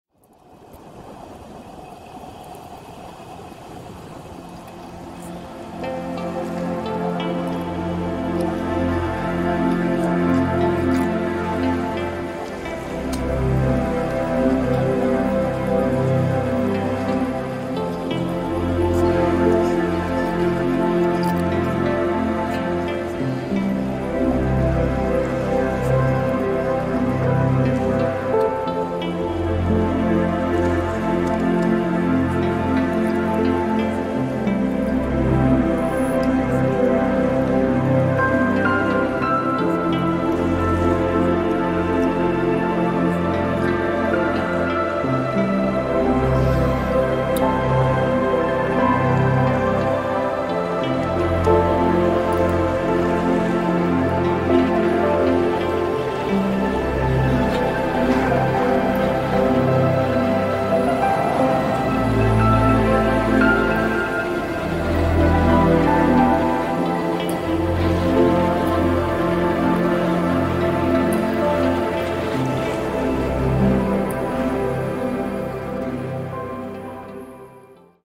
[HSK] Nierentrop → Dorlar
Mit dem Rennrad von Nierentrop nach Dorlar (Geschwindigkeit: 300%). Folgevideos